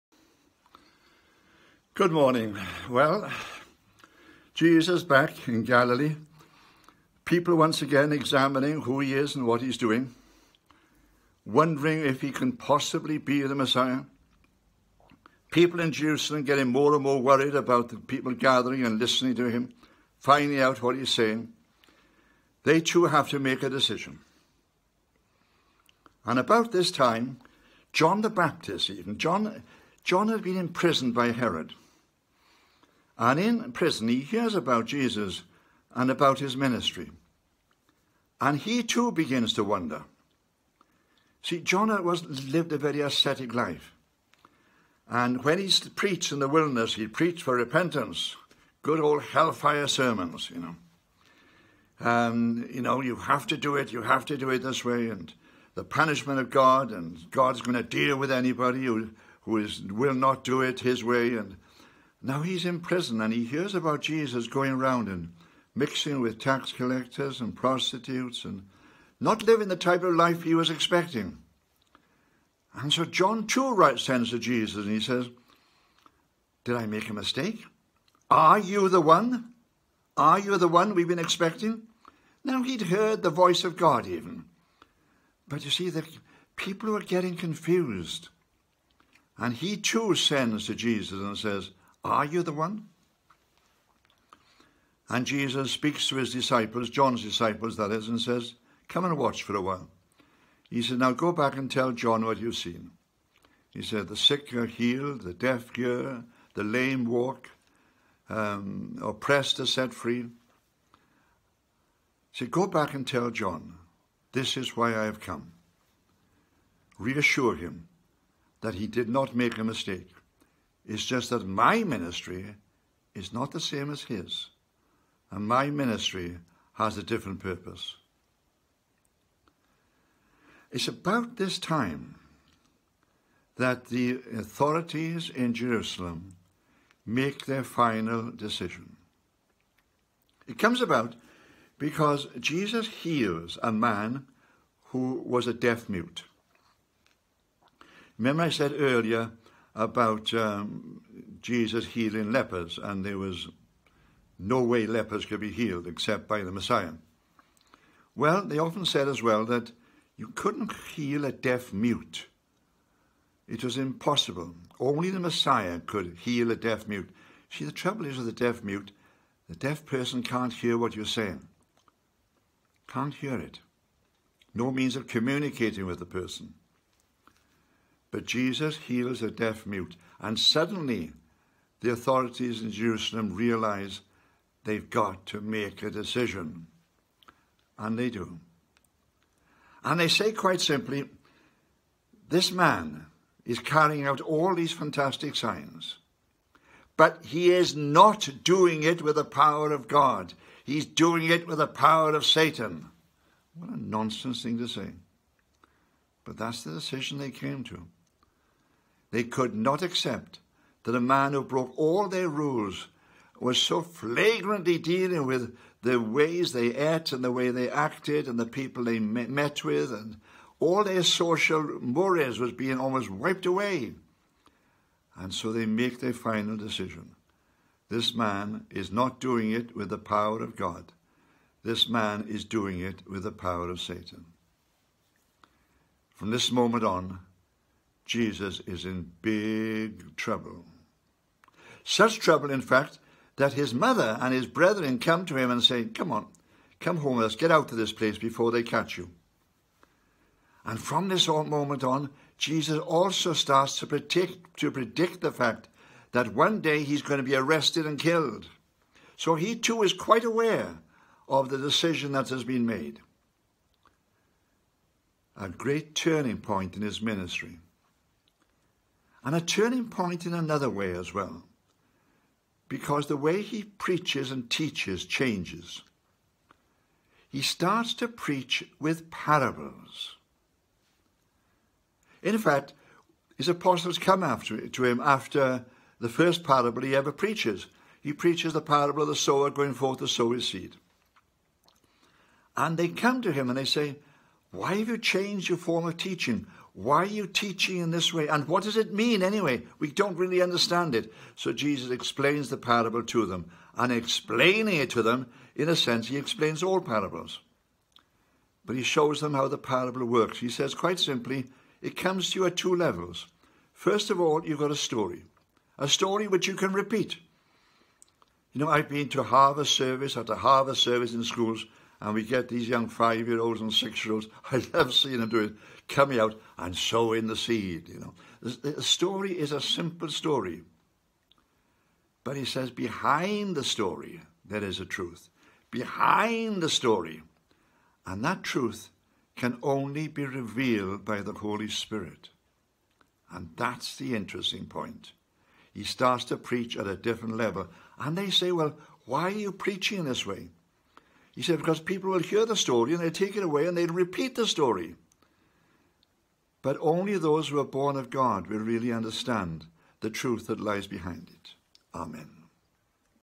Tuesday, 22nd September, 2020
Fr Roy Matthews continues his series on the writings of St Luke. Jesus had returned from Galilee, people were wondering if Jesus is the Messiah and even John the Baptist, in prison, sends to Jesus to ask him: are you the one? Jesus replies explaining that his role is different. The healing of a deaf mute, was considered to be something that only the Messiah could do and, when Jesus does this, the authorities accuse him of using the power of the devil. With his life now under threat, this is a turning point in Jesus ministry, and he begins to teach at a new level using parables: stories that can be retold but only understood in the light of the Spirit.